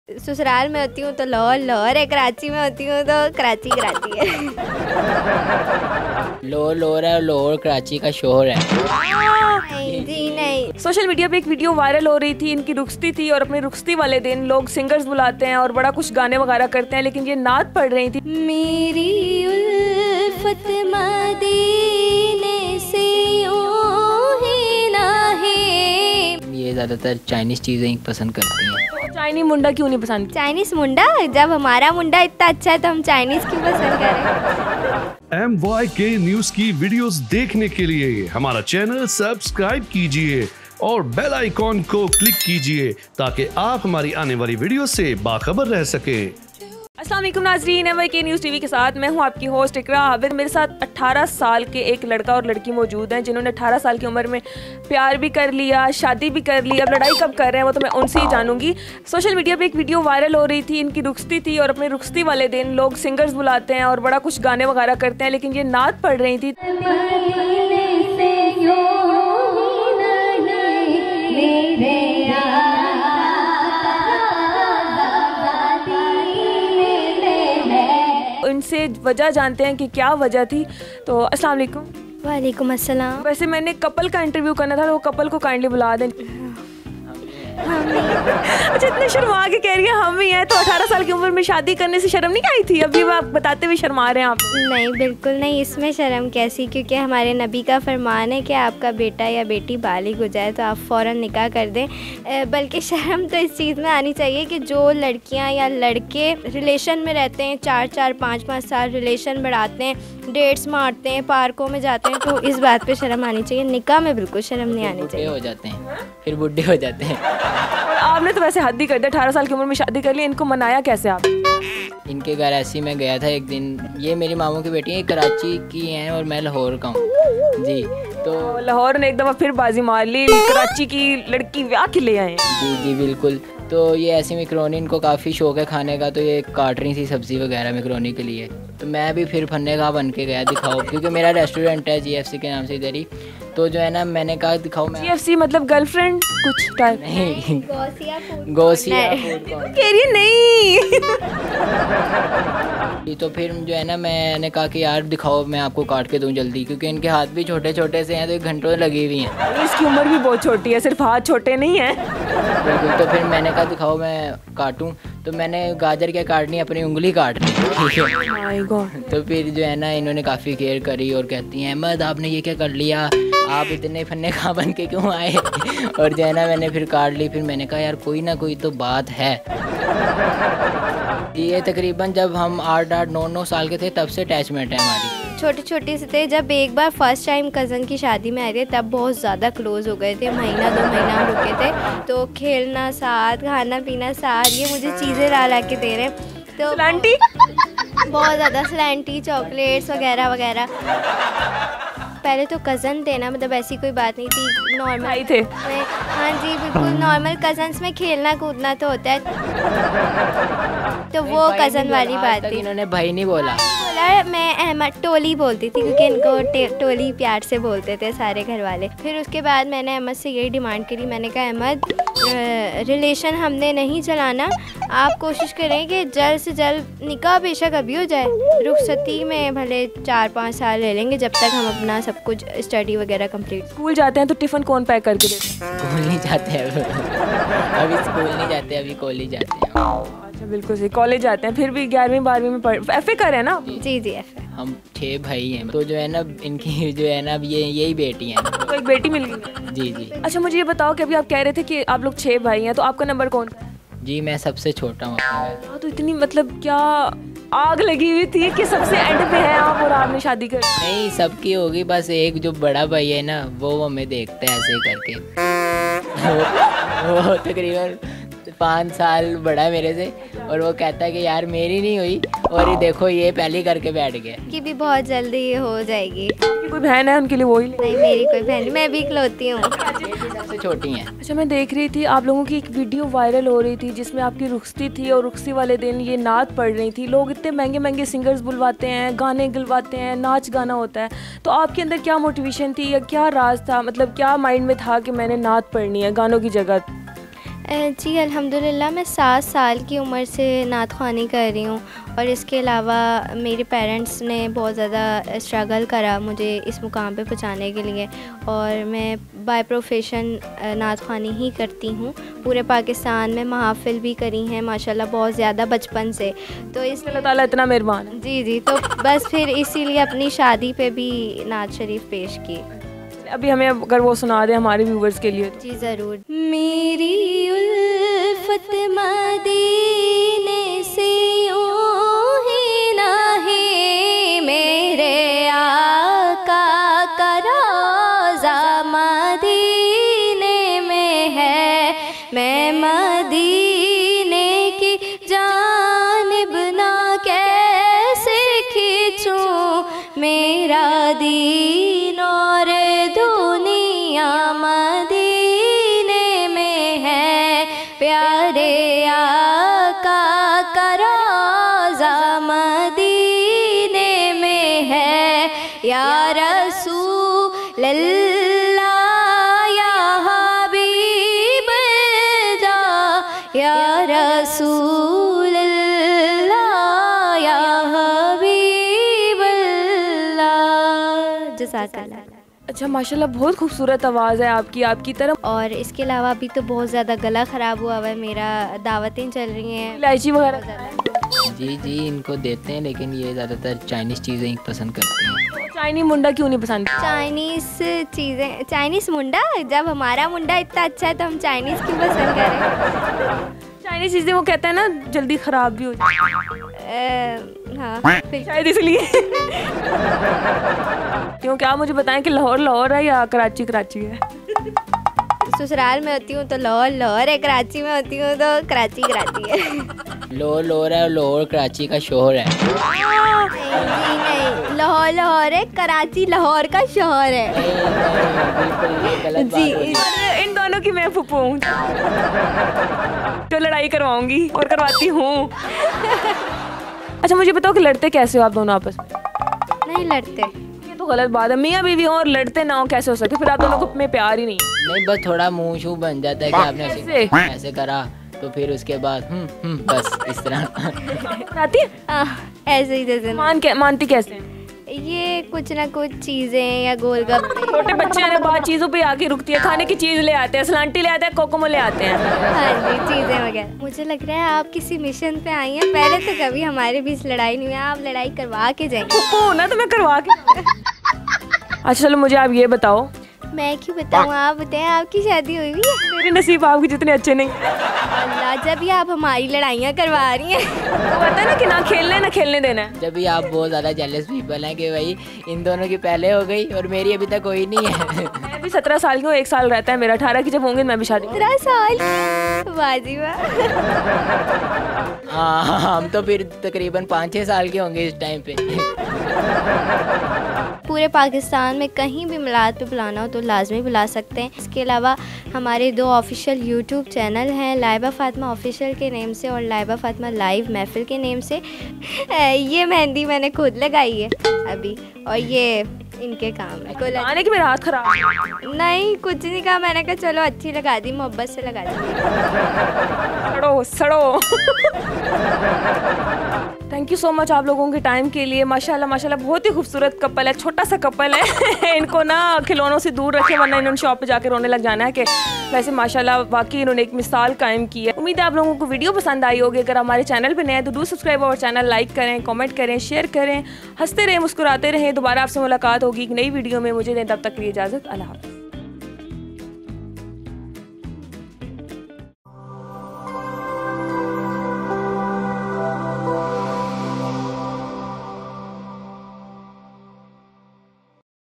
Barat Per Naat Parne Wala Viral Couple | Ahmed Aur Laiba Kay Sath Iqra Abid Ki ChatPati baten
MYK News TV anchor Iqra Abid interviewed 18-year-old Ahmed and Laiba, who got married and won the hearts of people by singing Naat instead of dancing and singing at their barat. Anchor Iqra Abid asked fun questions that brought smiles. Laiba Naat won hearts.